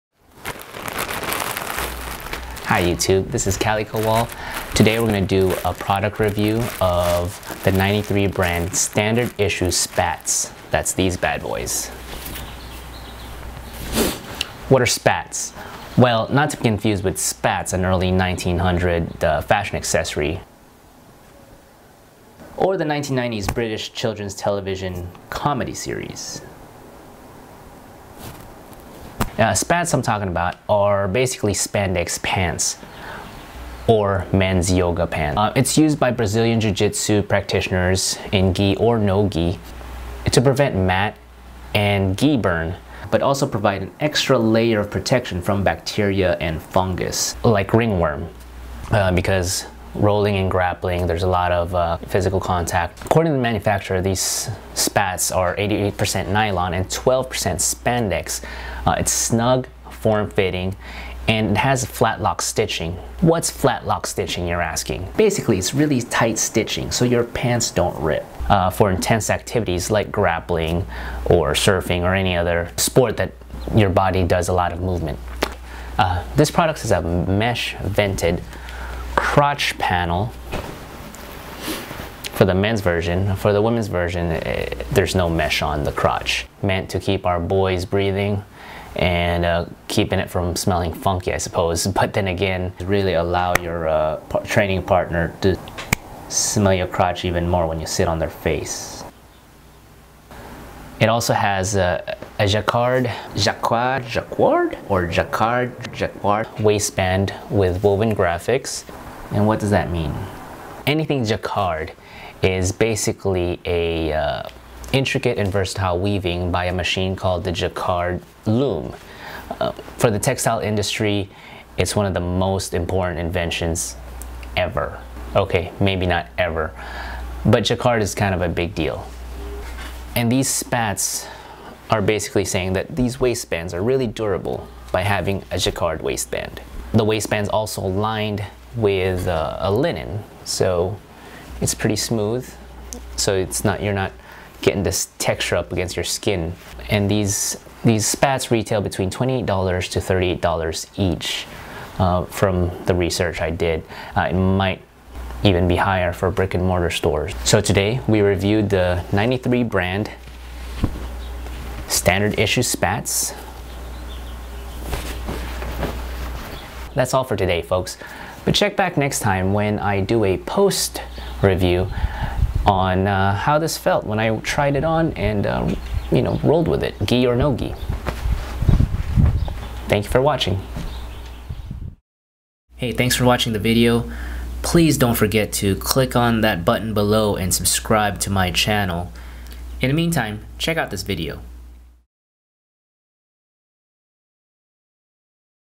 Product Review: 93 Brand "Standard Issue" Spatz
Received my 93 Brand "Standard Issue" Spatz in the mail. Sharing a quick product review out of the package. Hopefully, it helps people decide if they want to purchase it or not.

It's also meant to help people beginning in the world of BJJ.

I'll do a follow up review on how it fits and feels later.

Have you tried this product, tell us what you think too?